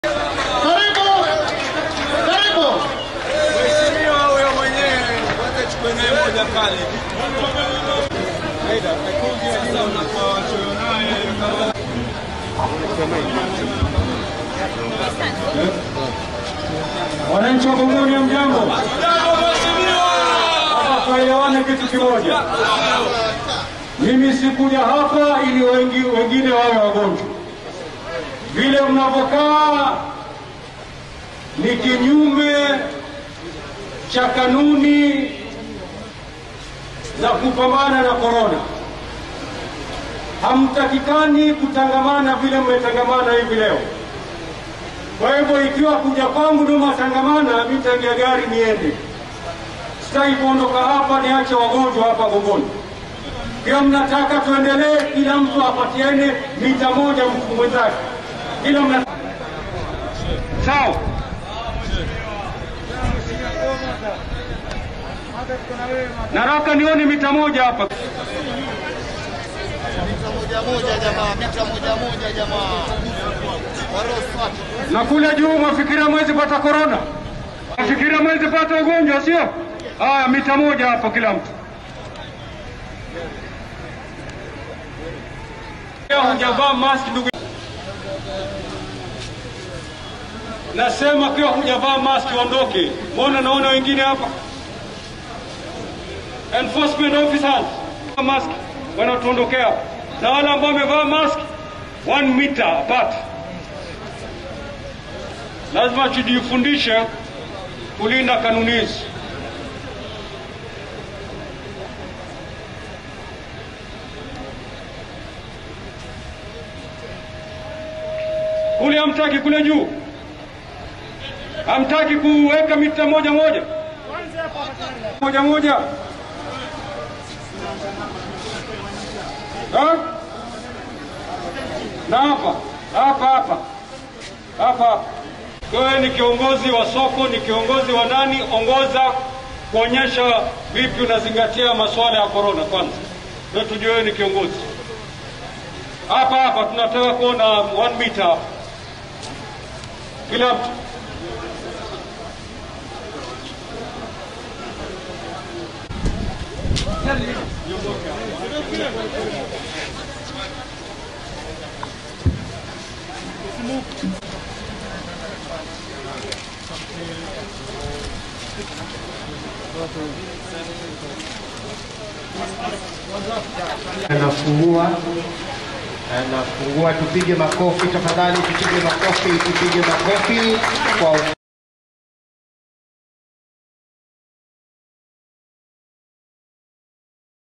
Kingi struggles with the crowd at the opening of a multi-million market in Gongoni, Magarini
Kilifi Governor Amason Kingi today nearly suspended the official launch of a multi-million market in Gongoni township Magarini after hundreds of residents faulted the COVID-19 rules.
The drama unfolded after hundreds of residents turned up at the market center some even not wearing masks and cheering causing panic to Kingi and his entourage.